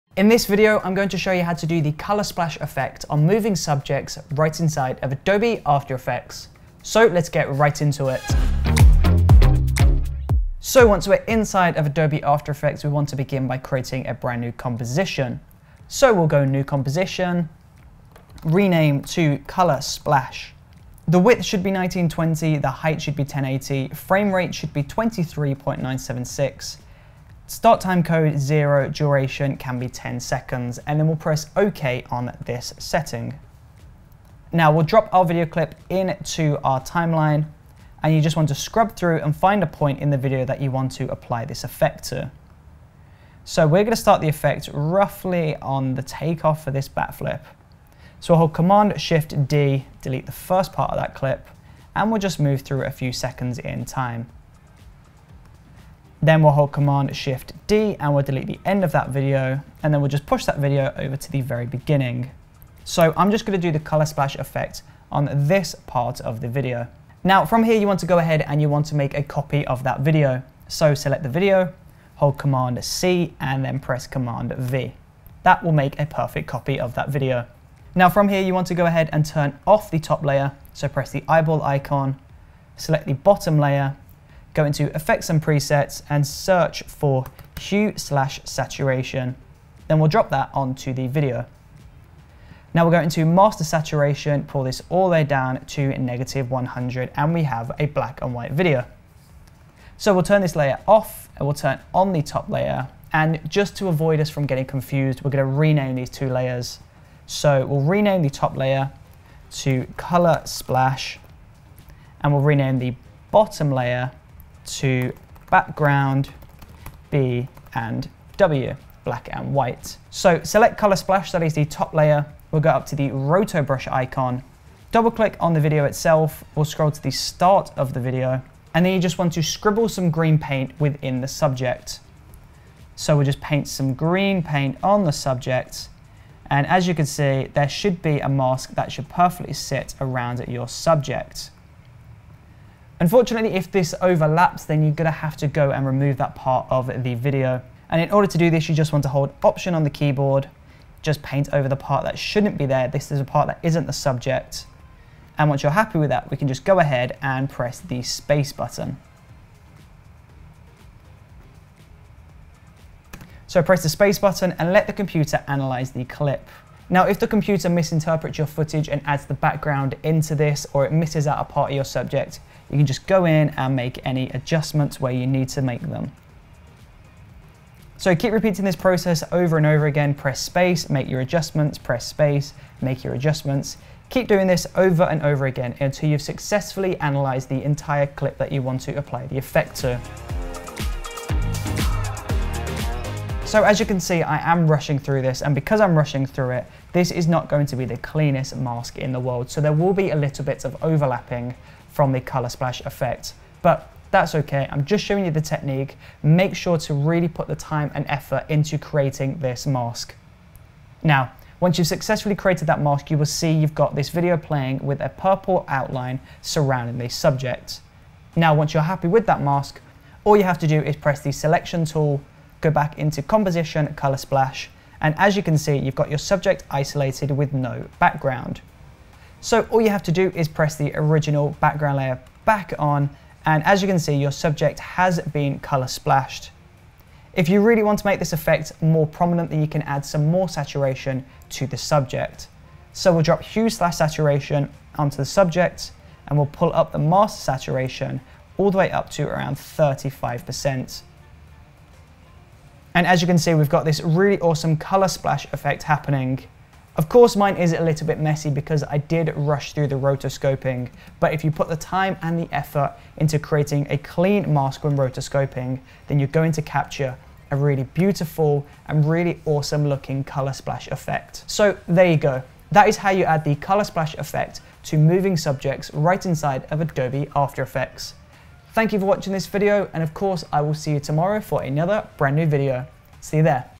Colour Splash Moving Subjects - After Effects
You can create a dynamic colour splash effect easily inside of Adobe Premiere Pro, but when applying the colour splash effect to moving subject, this gets more complex and requires a helping hand from After Effects. Find out how to isolate your subject and create an amazing colour splash effect in this video.